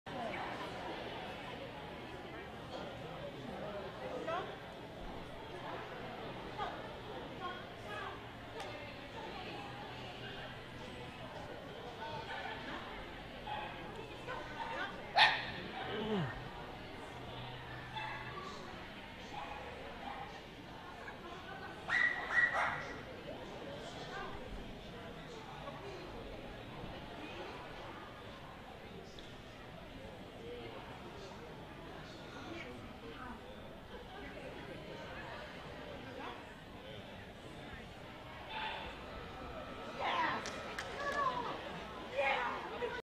Charm Agility Dog - AKC Trial Excellent Jumpers With Weaves 041010
Ratitatt Charmin Zboys (Charm) - Rat Terrier Running Excellent JWW Columbus Ohio Sports Barn Agility Trial 041010. Age 25 months.  Kathy Meyer Owner/Handler. Video by Agile Images